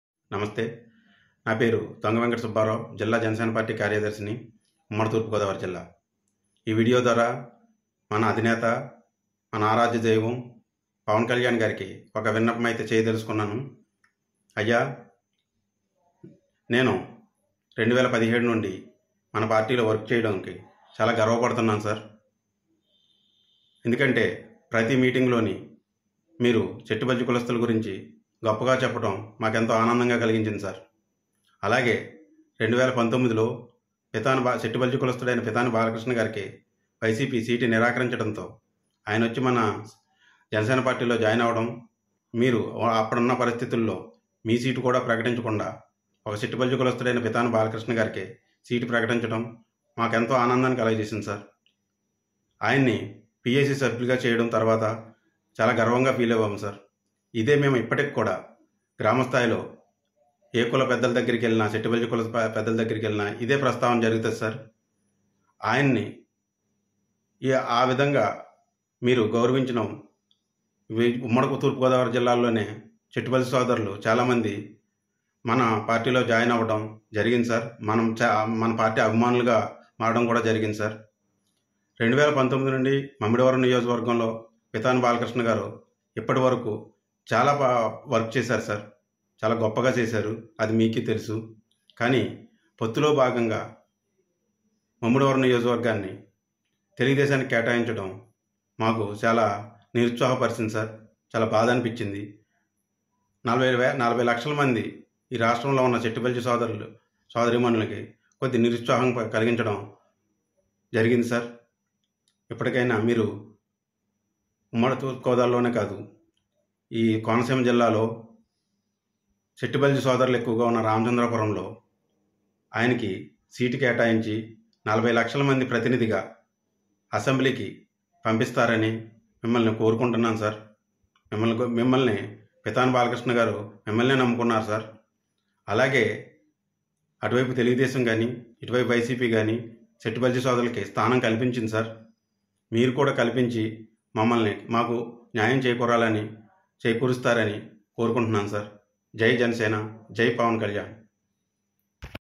40 లక్షల శెట్టిబలజలు వున్న ఈరాష్ట్రంలో, శెట్టిబలిజ సోదరుడు పితాని బాలకృష్ణ గారికి సీటు ప్రకటించాలని
40 లక్షల శెట్టి బలజలు వున్న ఈ రాష్ట్రంలో, శెట్టి బలిజ సోదరుడు పితాని బాలకృష్ణ గారికి రామచంద్రాపురం సీటు ప్రకటించాలని JanaSena Party అధినేత Pawan Kalyan గారికి విన్నపం.